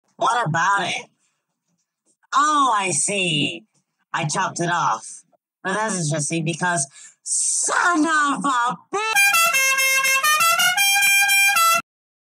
"Um... Sarcast... your arm." | Sarcast Voice Test (read desc)
My actual voice is not very fitting for Sarcast so I pitched it up by 2 tones in Audacity and pitched it back down by 2 tones in the NewTone plugin for FL Studio.

Results are not final just because my sister said I sound like friggin' Alvin!

Sorry, kids. This is not a Gacha video about me farting on someone. Go back to the shadows from whence you came. I f***ing love Nimona.